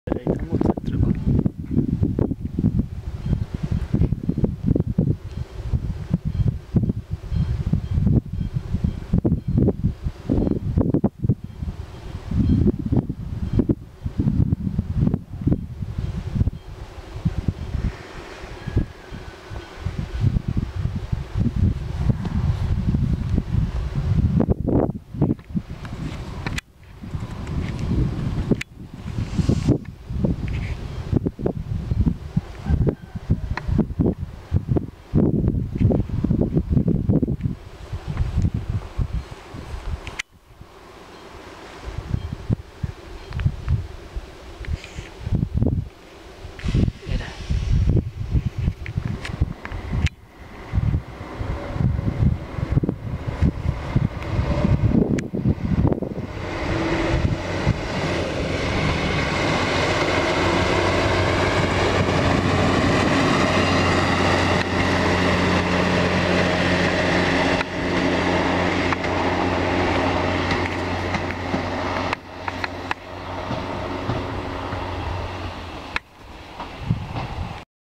810 195-8 odjezd Temelin os18211
Odjezd os.vlaku 18211  v podání krabičky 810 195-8 ze žst. Temelín .
Točeno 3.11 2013 foťákem Canon PowerShot A2300HD